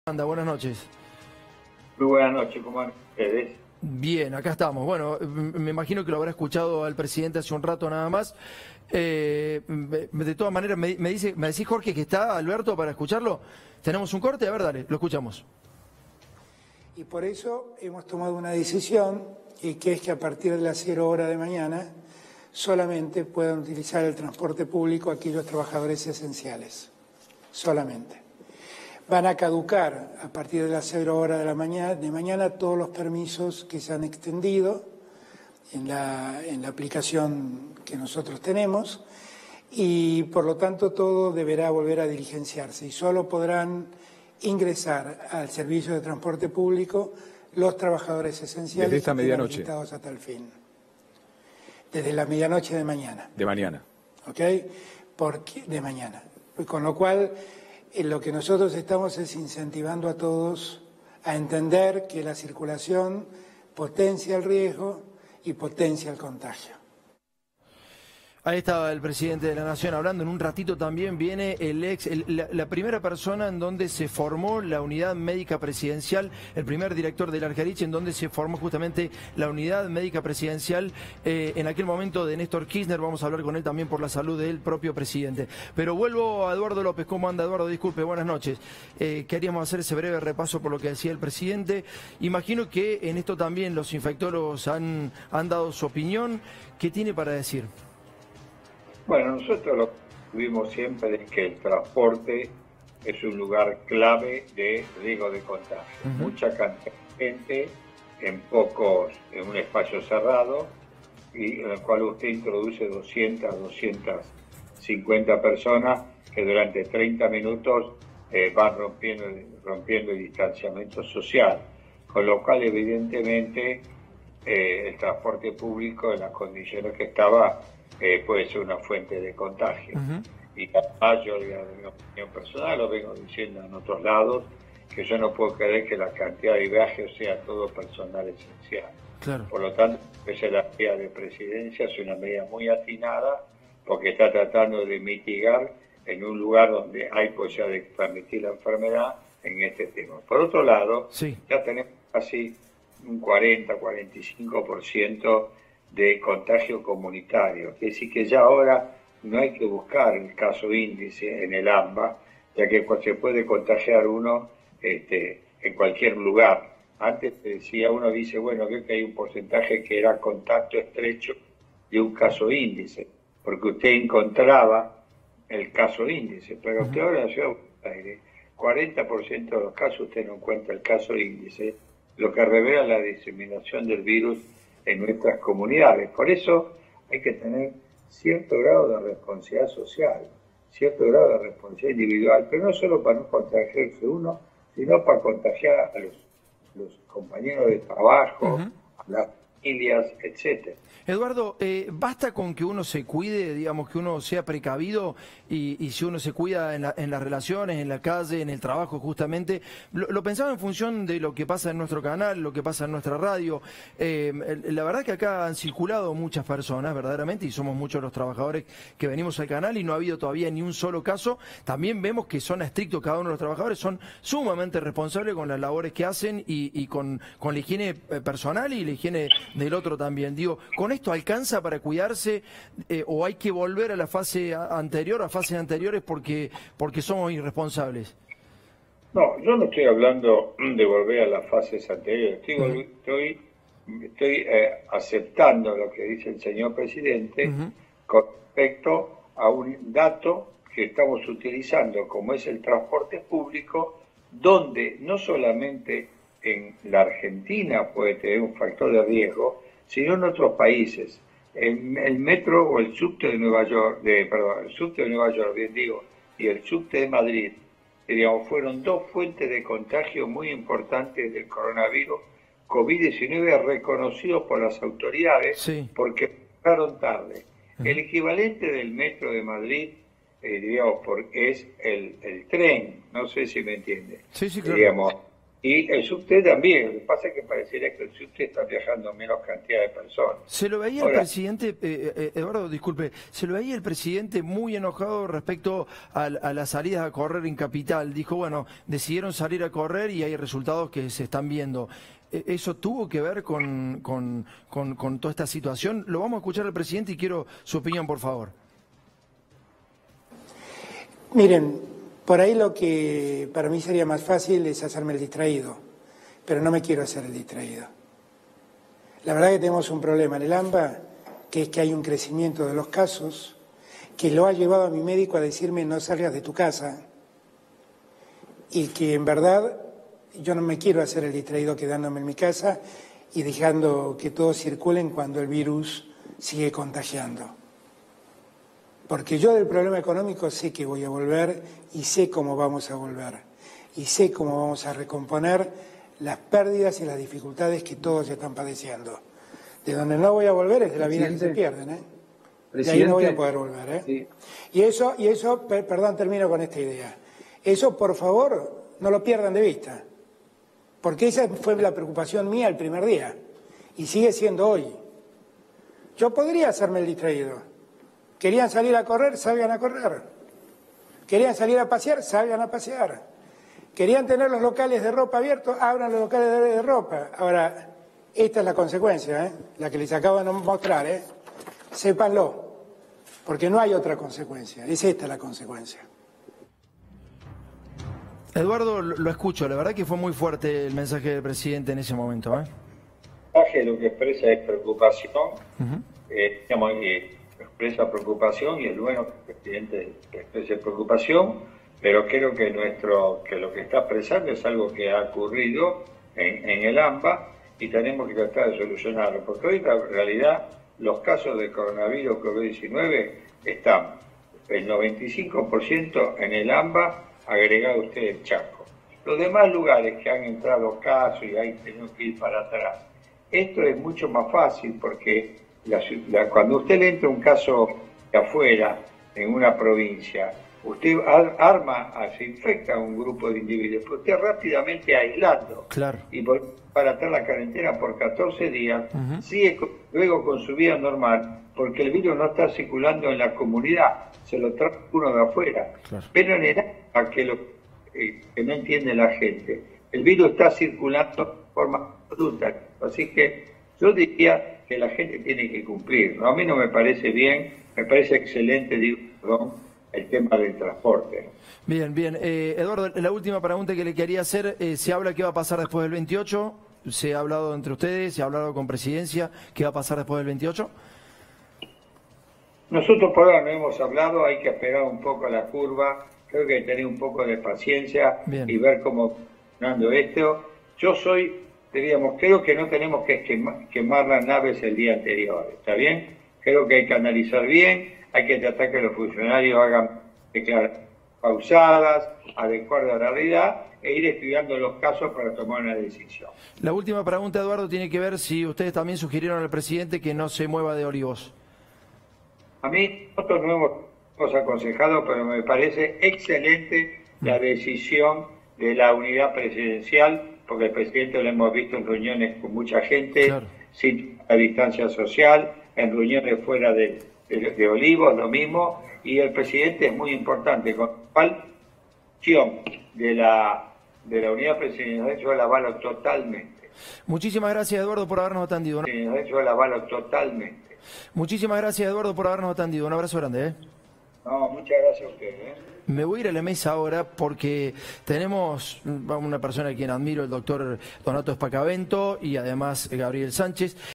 Esta medianoche caducan todos los permisos de circulación
El transporte es un lugar clave de contagio, y la circulación sin distanciamiento social hace que aumenten los casos.